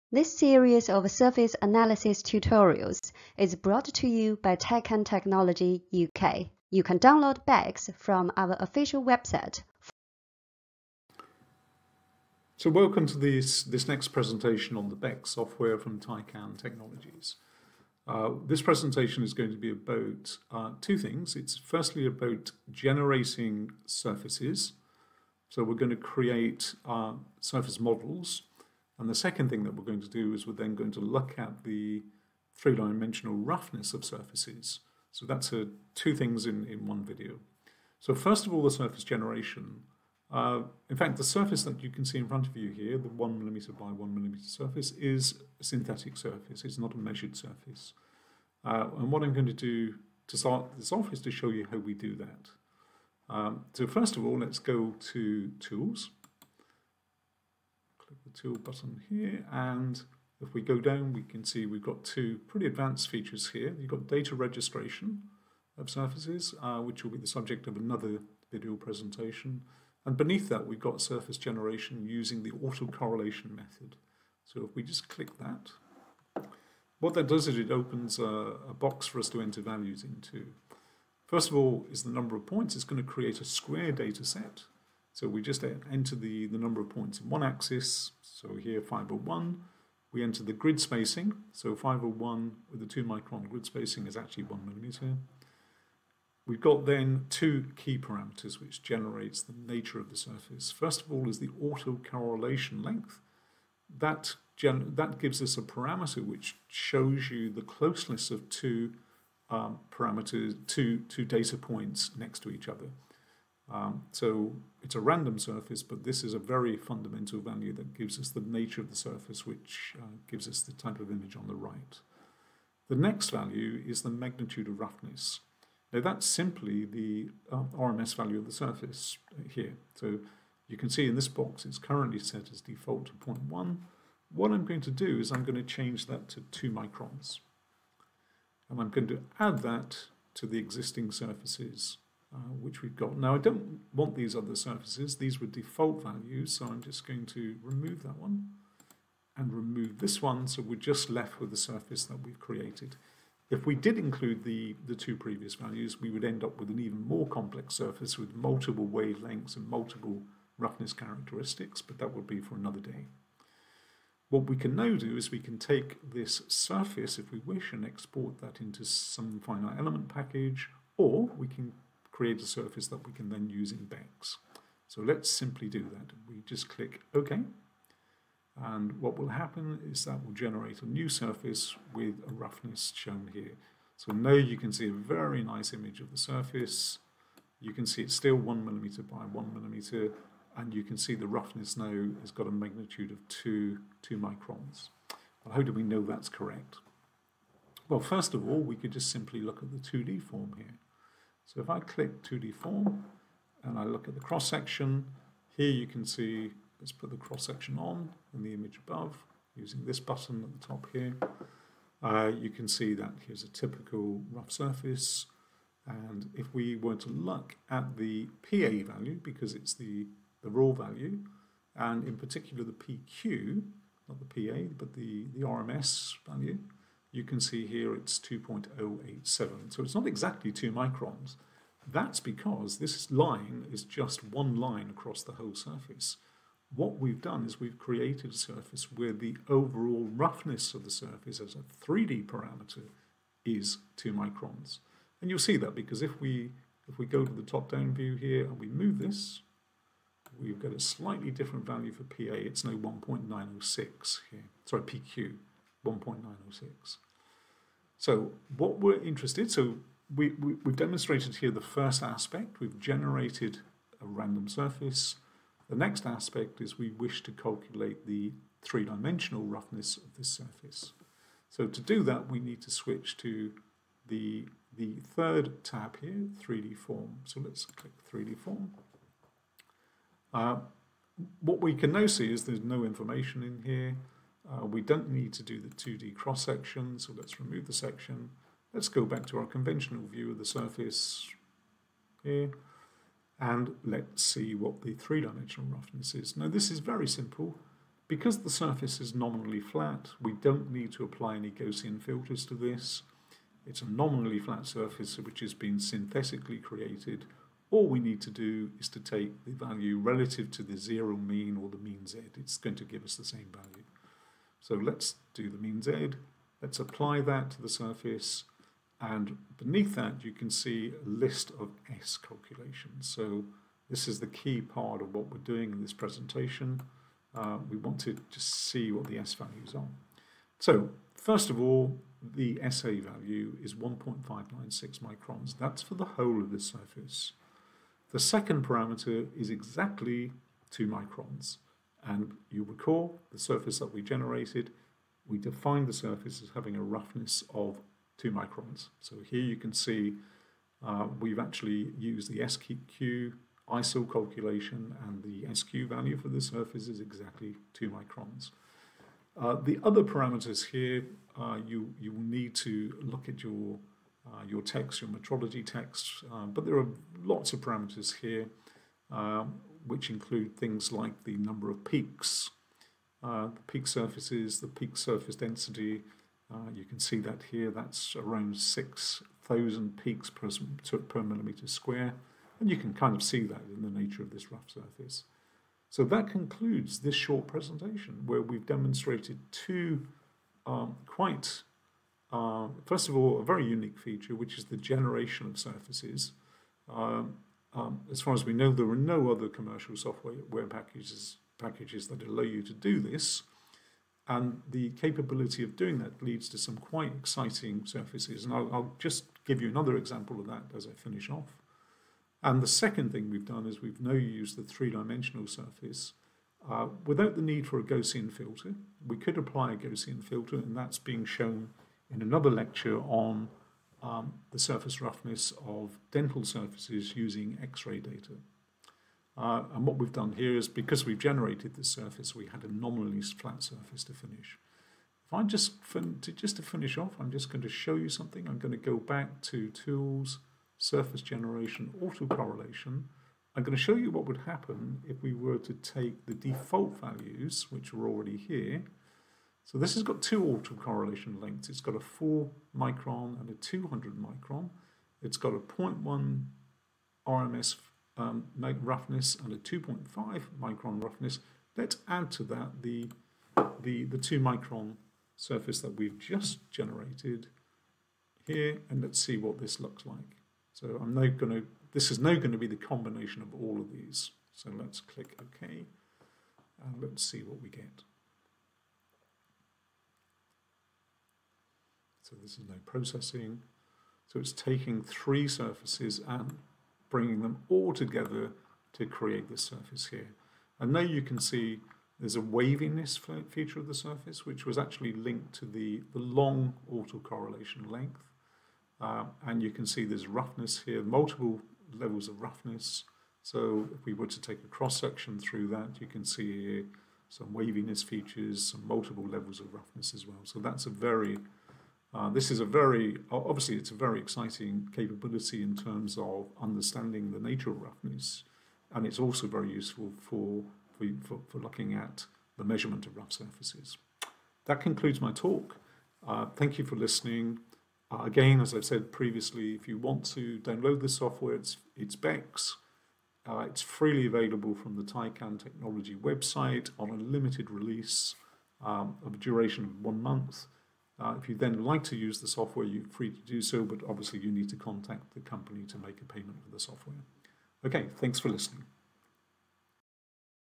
BEX Tutorial 14 - 3D Surface Generation and Gaussian Filters
In this tutorial we explain how to use autocorrelated surface generation to develop realistic surfaces, and then evaluate their 3D roughness using the S series parameters.